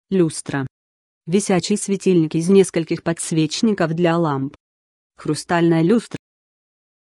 ЛЮСТРА - что это такое? значение и описание
Хочешь знать что значит ЛЮСТРА? тогда слушай до конца. n n -ы, ж. Висячий светильник из нескольких подсвечников для ламп. Хрустальная л. || прил. люстровый, -ая, -ое. n n Больше слов